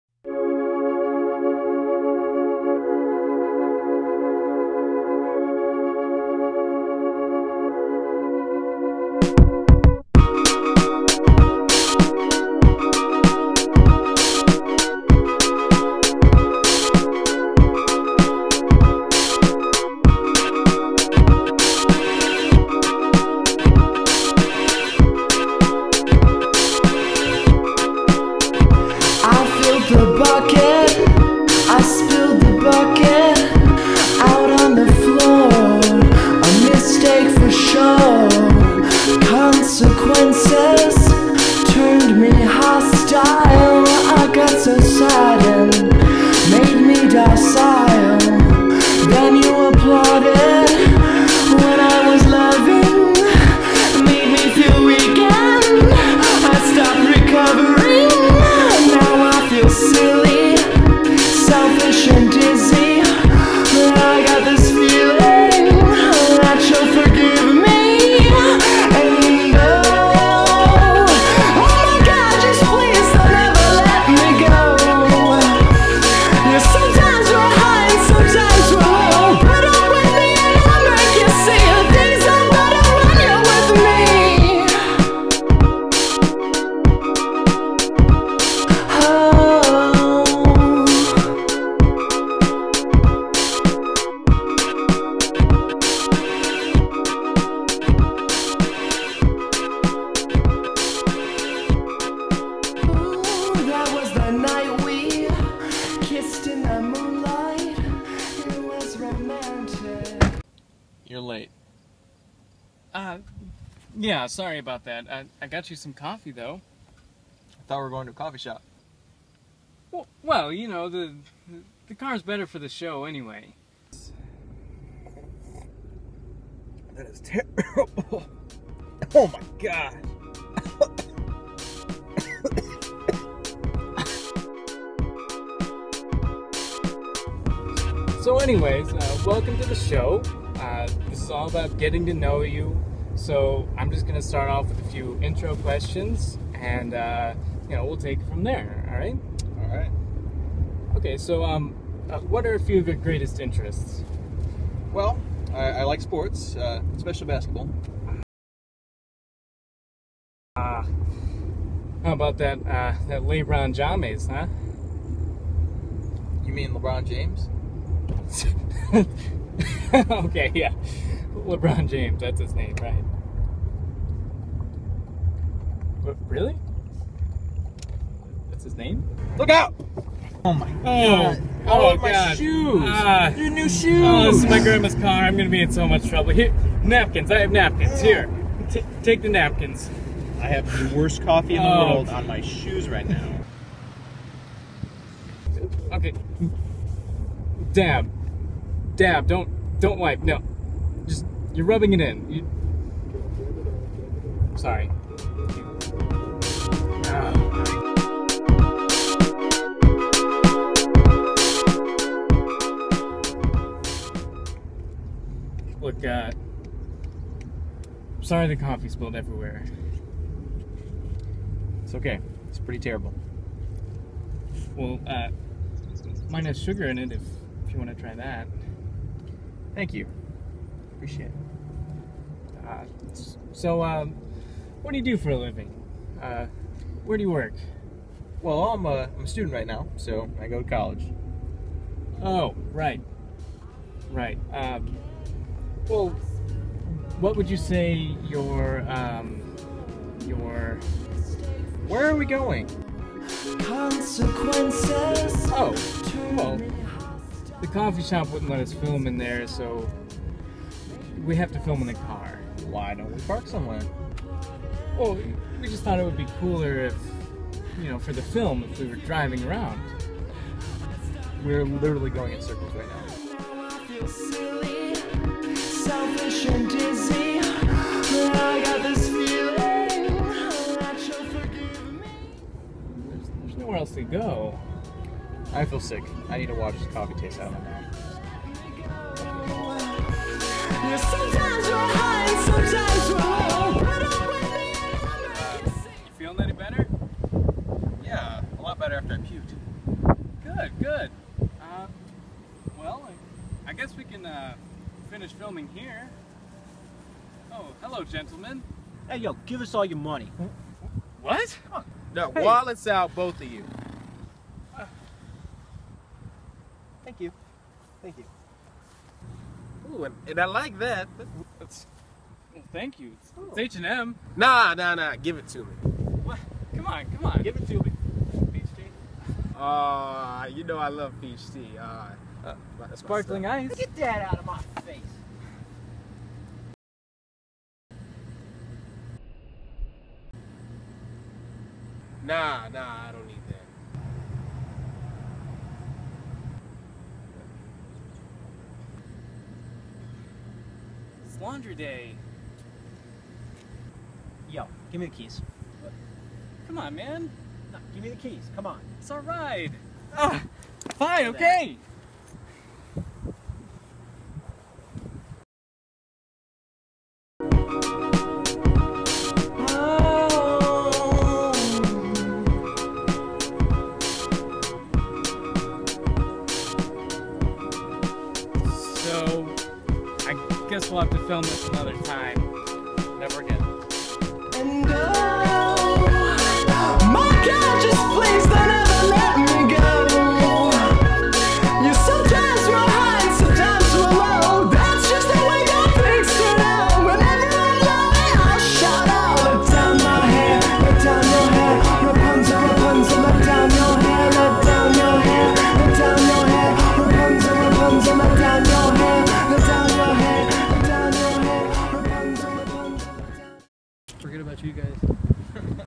Crappy Coffee With Jeff Cox
Jeff's wannabe hipster talk show goes awry.

This was all shot on an iphone so excuse the poor sound quality and the occasional crappy shot!